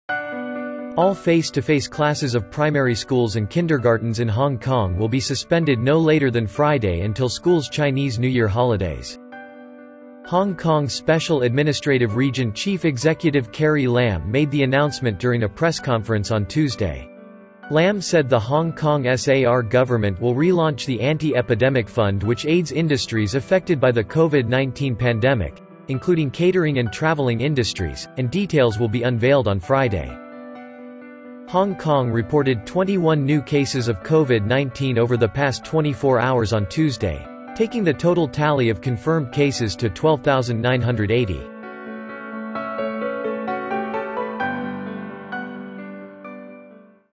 Hong Kong to suspend face-to-face classes of primary schools, kindergartens
Hong Kong to suspend face-to-face classes of primary schools, kindergartens due to COVID-19